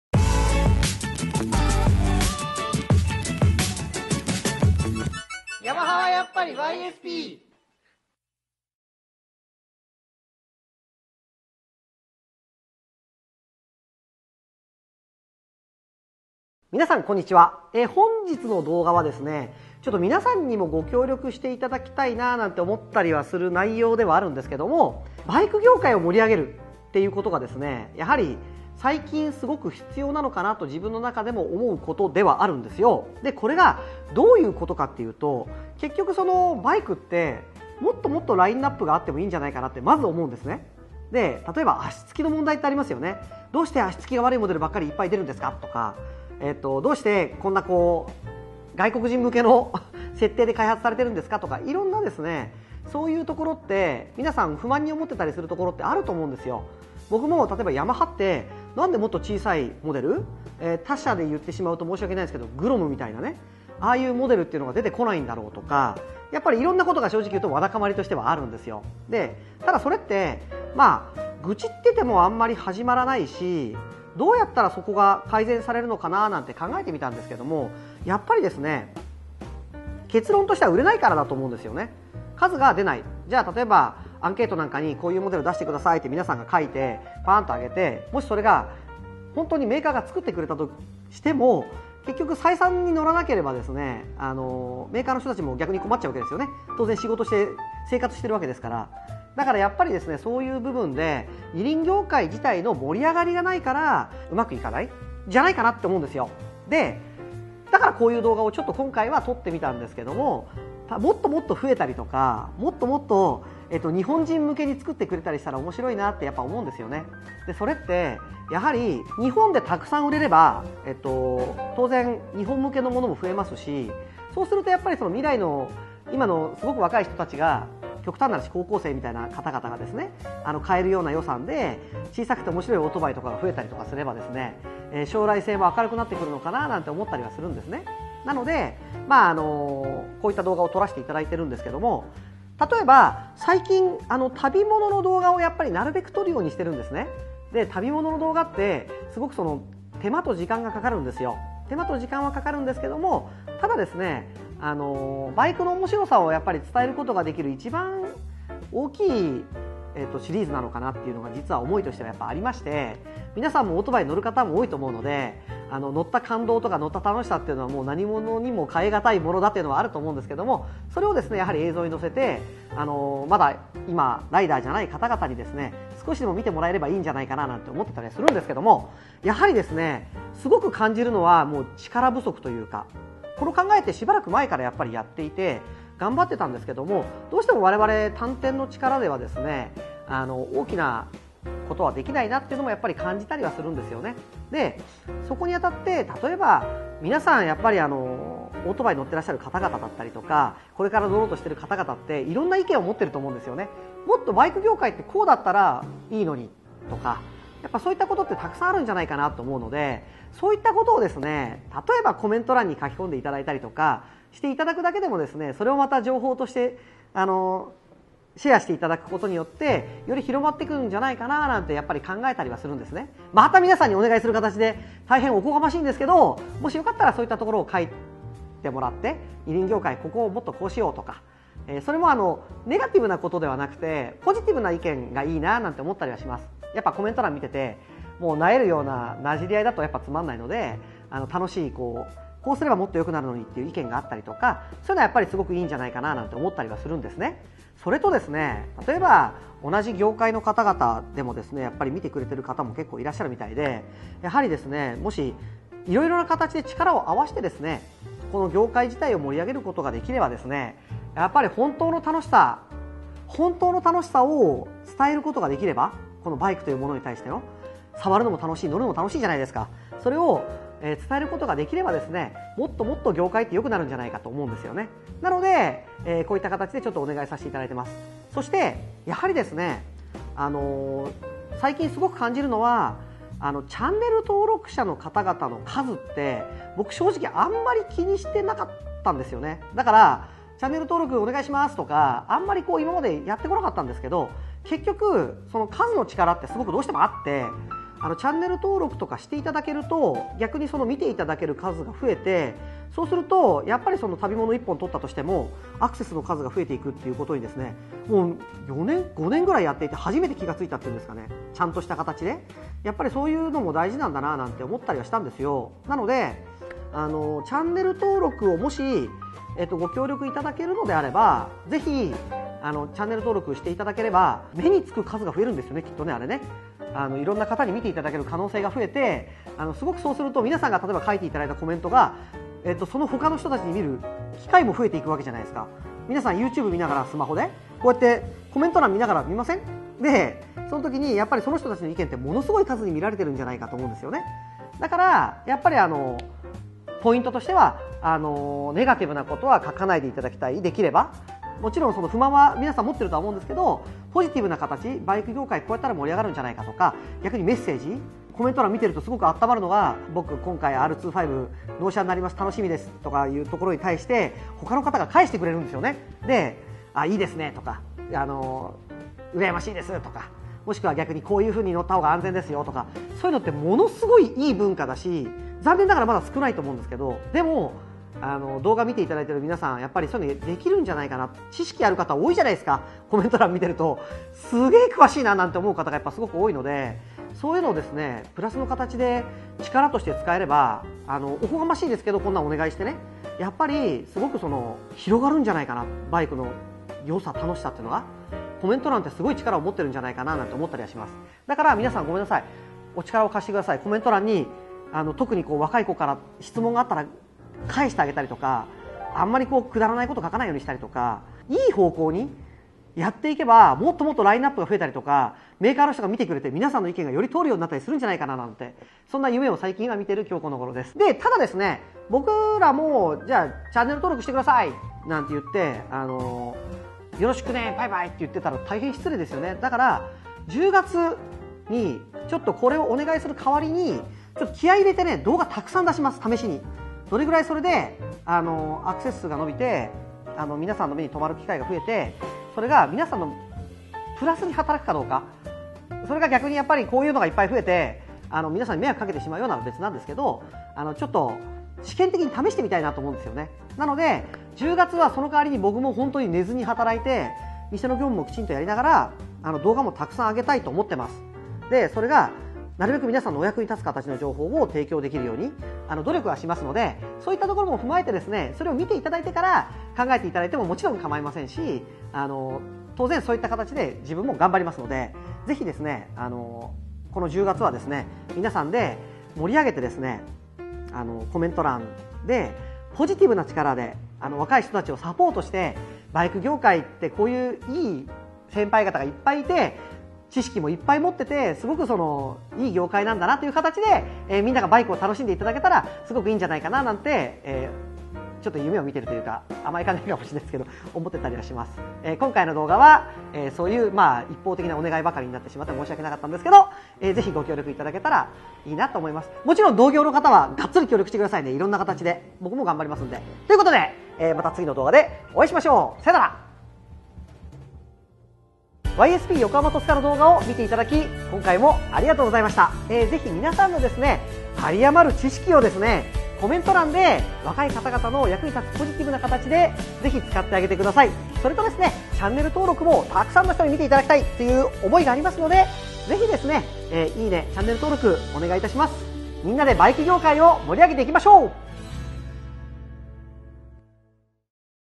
フリートーク・みんなでバイクを盛り上げよう！byYSP横浜戸塚
●コメント欄にご意見求めます！皆様お気軽にどうぞ～♪
「どうすれば、バイクはもっと盛り上がるのか？！」

こんにちは！YSP横浜戸塚です。　今回の動画は、「みんなでバイクを盛り上げよう！」という内容になります。またコメントを募集しますので、色々とご意見下さい！　頂いたご意見は、メーカーや私達ディーラー、お付き合いのあるメディアの方に伝える事で、より二輪業界が発展すればと考えています。
それでは、チェック宜しくお願いします~

営業時間：AM10:00～PM8:00　定休日：毎週水曜日

▼YSパンダグッズ
▼YSパンダLINEスタンプ
▼タイアップやPRのご相談